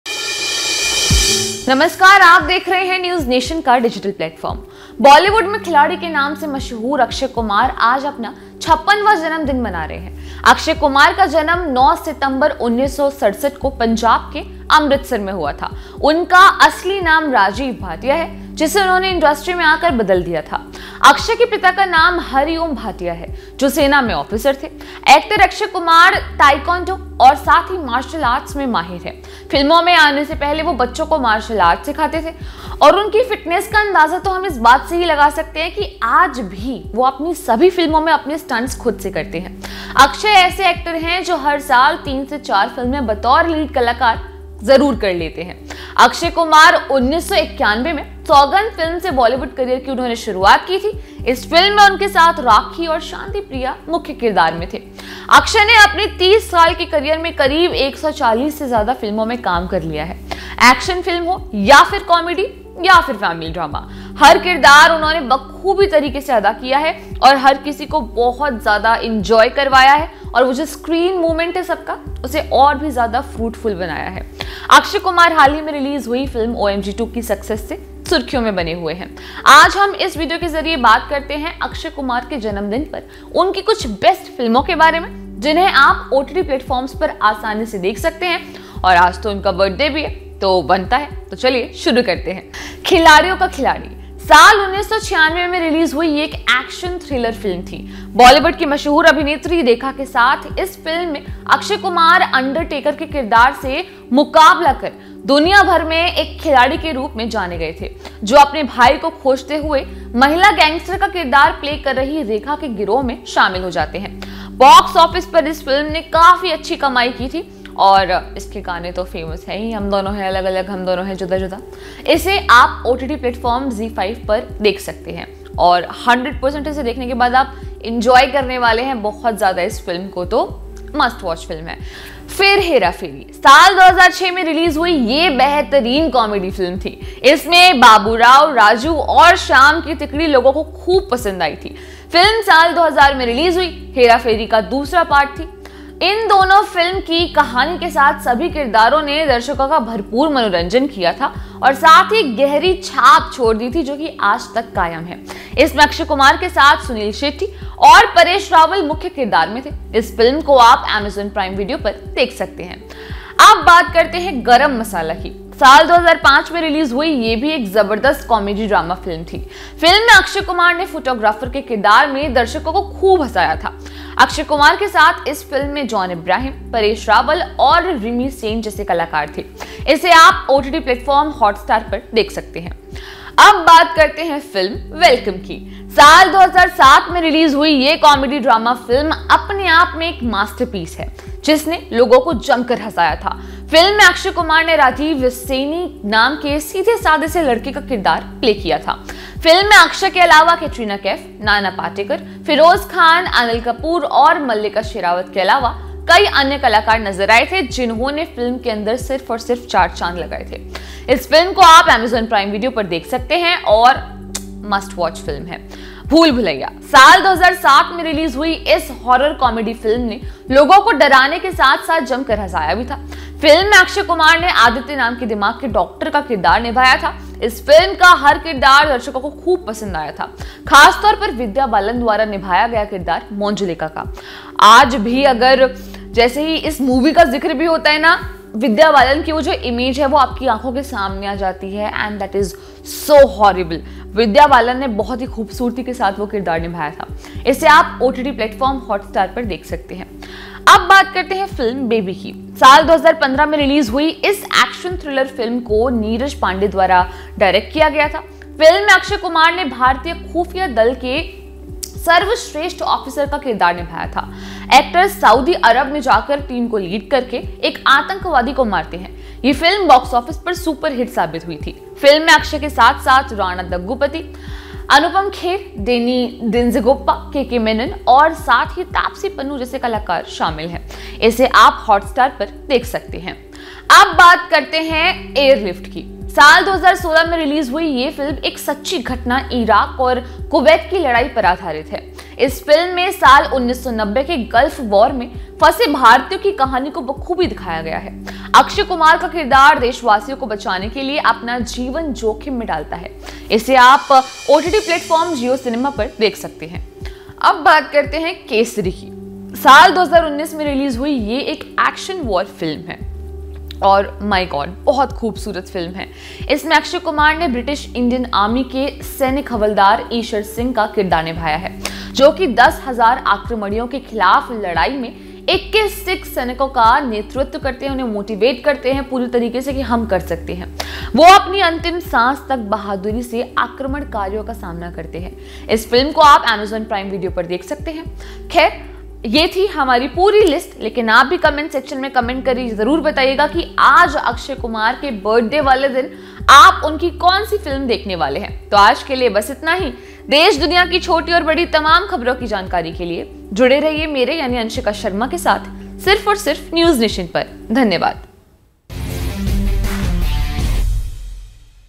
Akshay Kumar Birthday Special:'Saugandh' से किया डेब्यू,फिर इन फिल्मों से बने Bollywood के 'Khiladi'
बॉलीवुड में खिलाड़ी के नाम से मशहूर अक्षय कुमार आज अपना 56(छप्पन)वां जन्मदिन मना रहे हैं। अक्षय कुमार का जन्म 9 सितंबर 1967(उन्नीस सौ सड़सठ) को पंजाब के अमृतसर में हुआ था। उनका असली नाम राजीव भाटिया है, जिसे उन्होंने इंडस्ट्री में आकर बदल दिया था। अक्षय के पिता का नाम हरिओम भाटिया है, जो सेना में ऑफिसर थे। एक्टर अक्षय कुमार ताइक्वांडो और मार्शल आर्ट्स में भी माहिर हैं। फिल्मों में आने से पहले वो बच्चों को मार्शल आर्ट्स सिखाते थे...